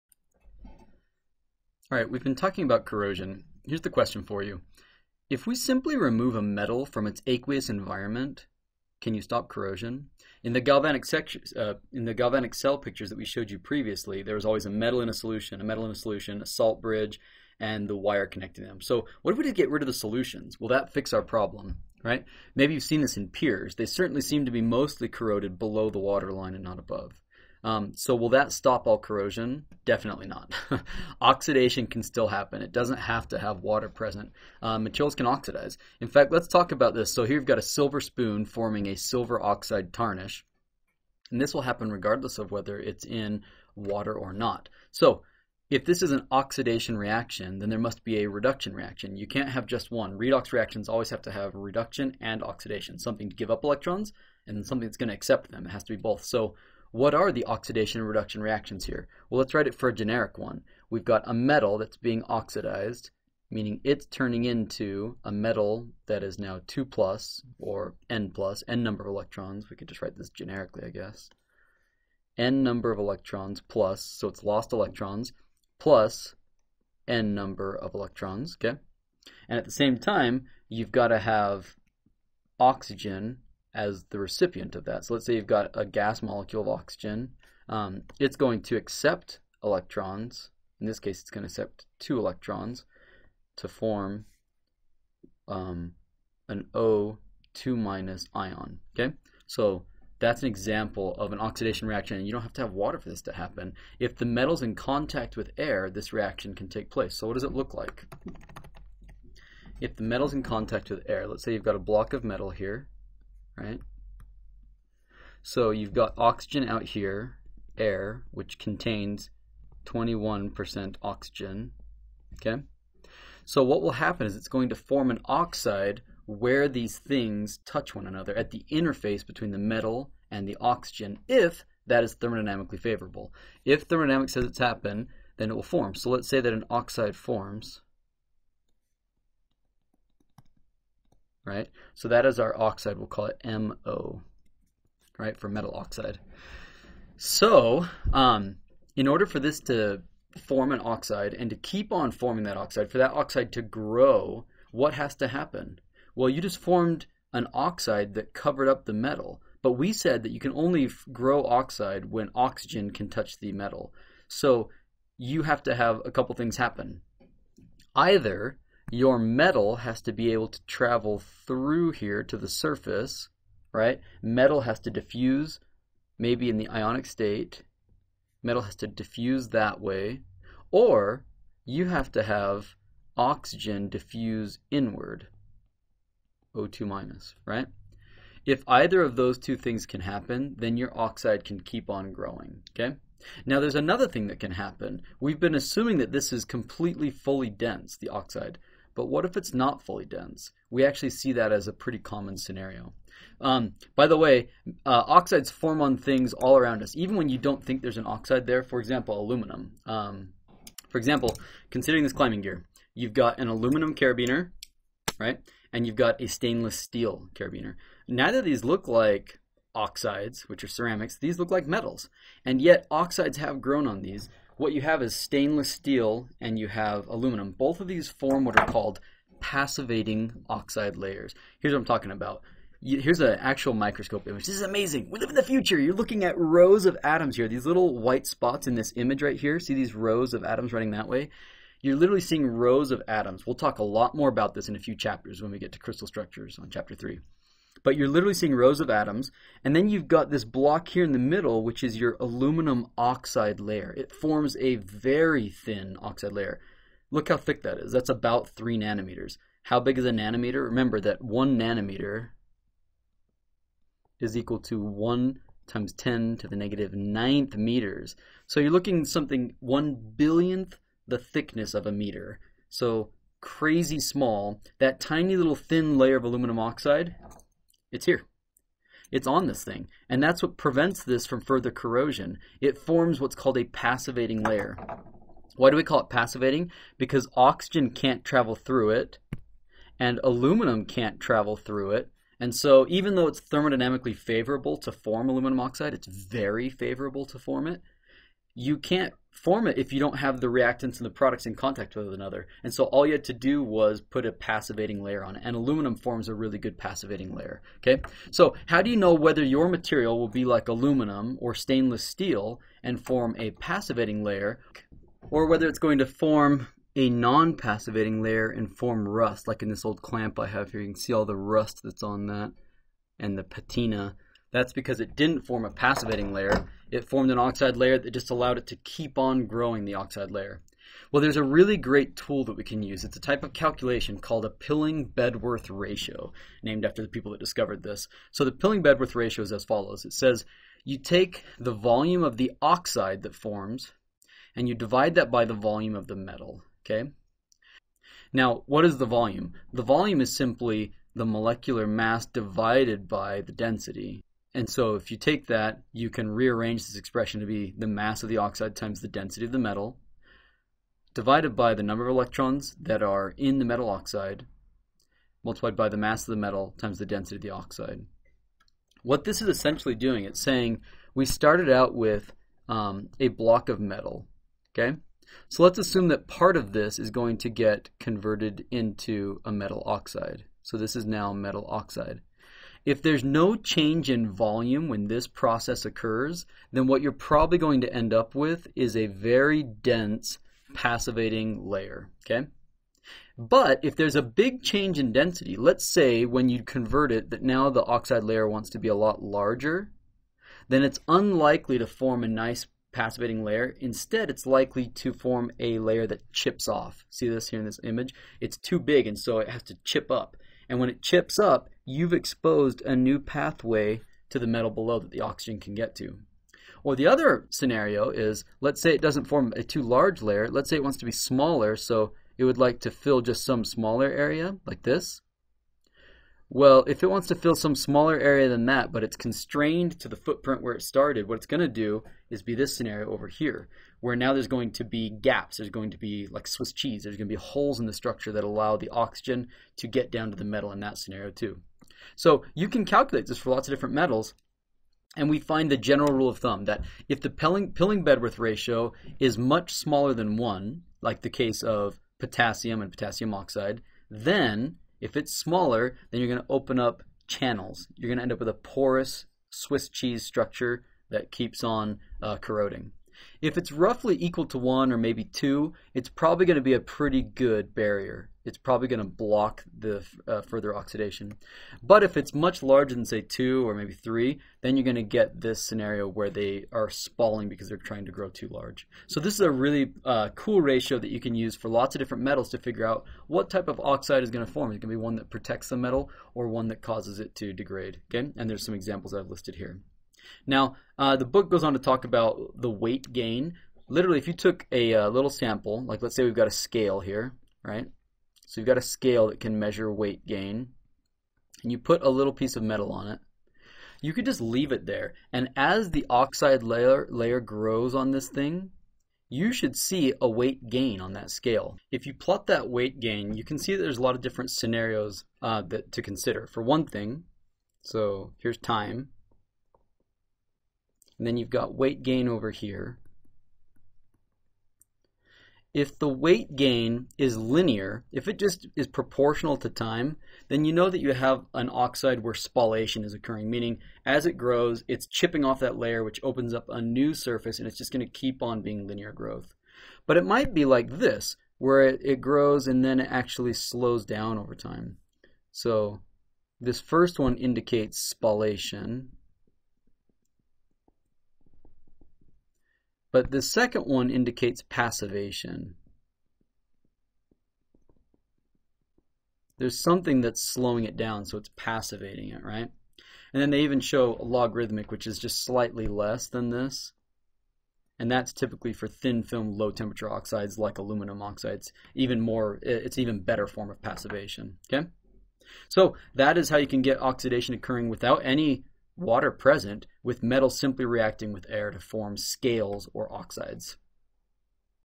How metals are able to oxidize in air
You've probably left a bike out in the rain and discovered some unwanted rust on it afterwards. Can metals oxidize in the air without water though? Yes! It depends on a couple of conditions being met. In this video we explain the Pilling-Bedworth ratio for calculating which metals will oxidize away and which will form passivating oxides that prevent further oxidation.